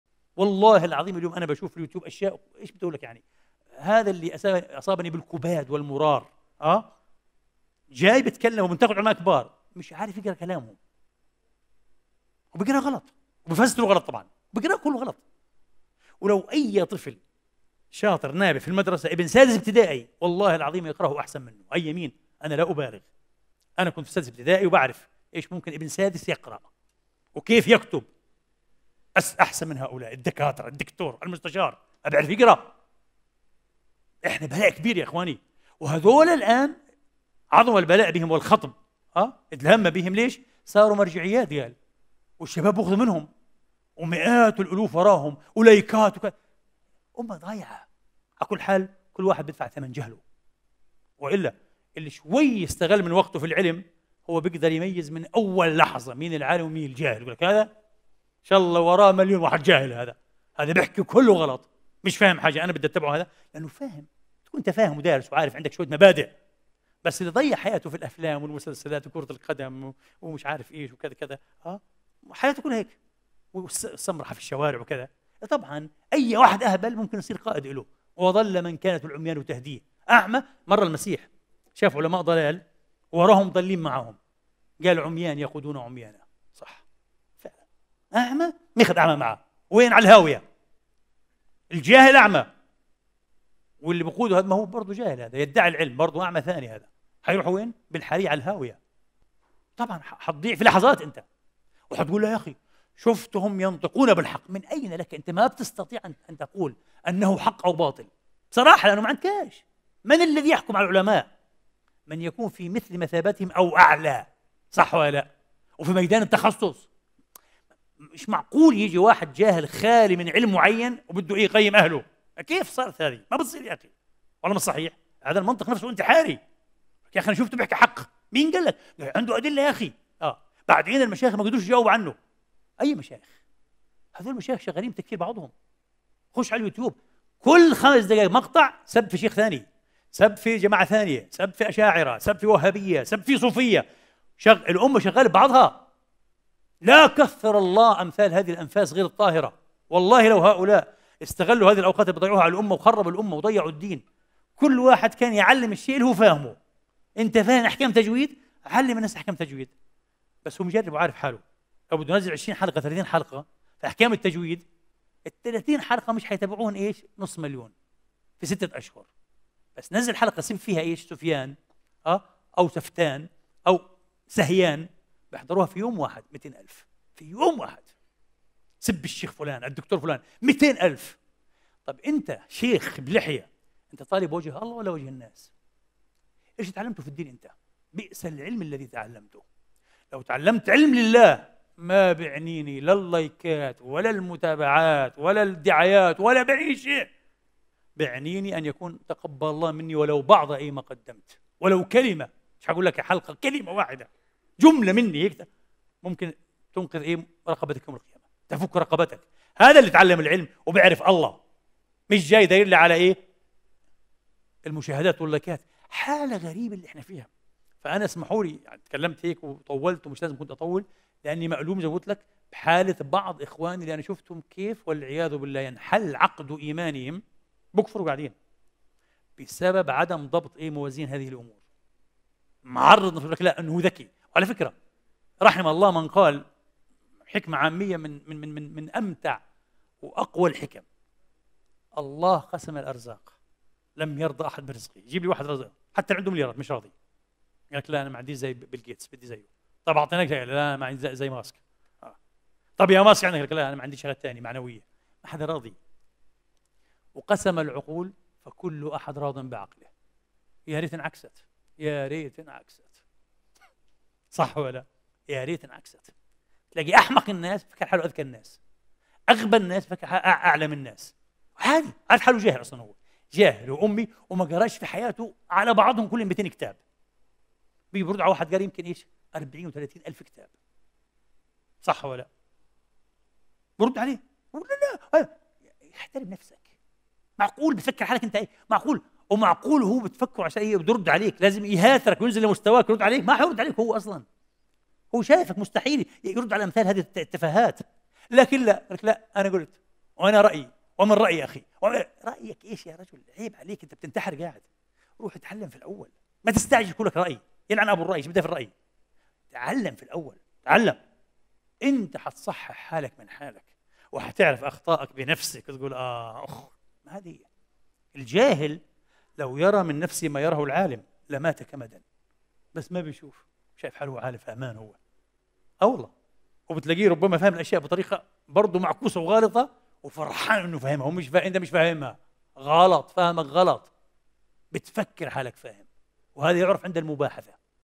جديد 😥 أشياء يراها الدكتور على اليوتيوب وتصيبه بالكباد والمرار 😥 عدنان إبراهيم
من عصر الجهالة - للدكتور عدنان إبراهيم

للوصول إلى حسابات فضيلته الرسمية:
adnanibrahim#
adnan_ibrahim#